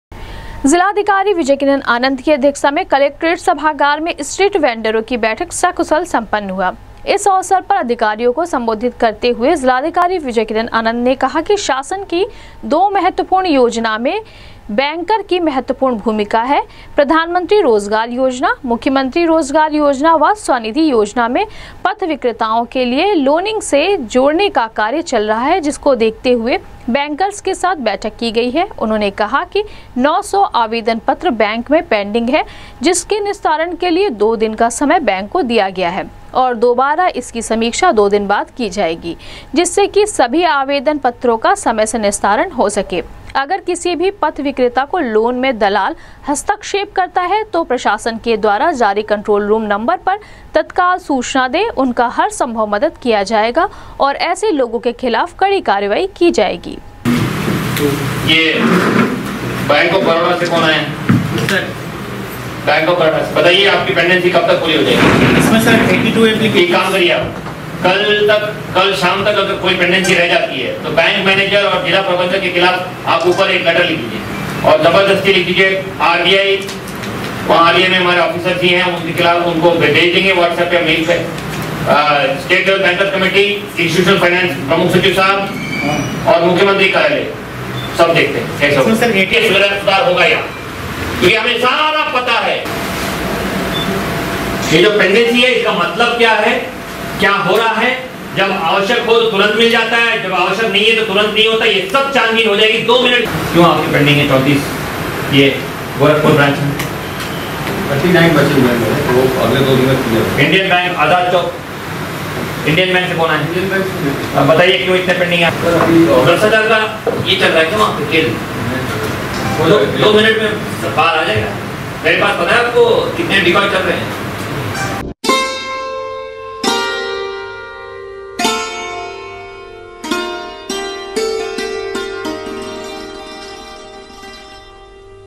किसी भी तरह की समस्या होने पर स्ट्रीट वेंडर कंट्रोल रूम के नम्बर पर कर सकता है शिकायत, तत्काल होगी
स्ट्रीट वेंडरों की बैठक जिलाधिकारी विजय किरन आनंद की अध्यक्षता में सकुशल हुआ संपन्न,कहा, नौ सौ पेंडिंग एप्लिकेशन का बैंक दो दिनों के अंदर करे निस्तारण,किसी भी तरह की समस्या होने पर स्ट्रीट वेंडर कंट्रोल रूम के नम्बर पर कर सकता है शिकायत, तत्काल होगी कार्यवाही।।